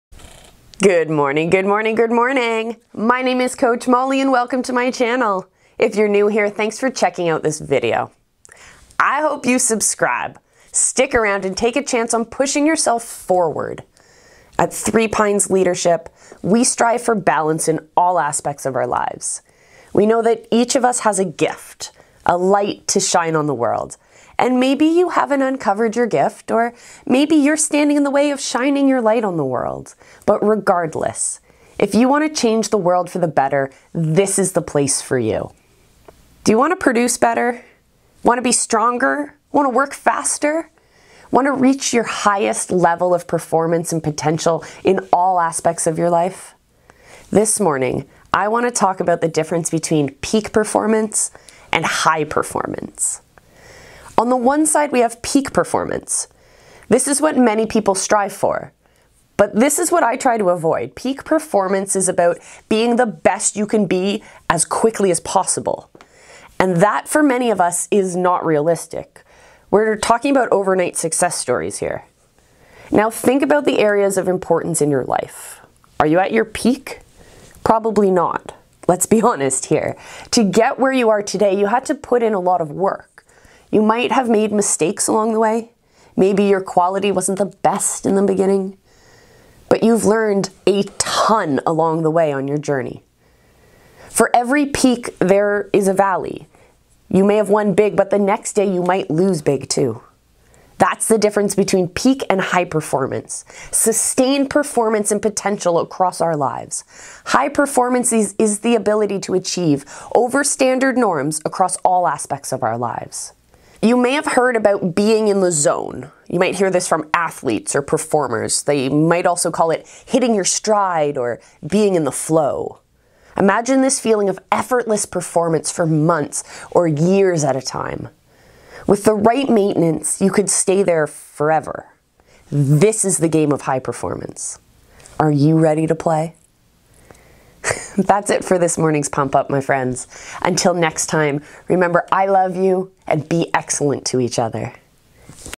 Is that the best you can do?
Today we discuss the difference between Peak Performance and High Performance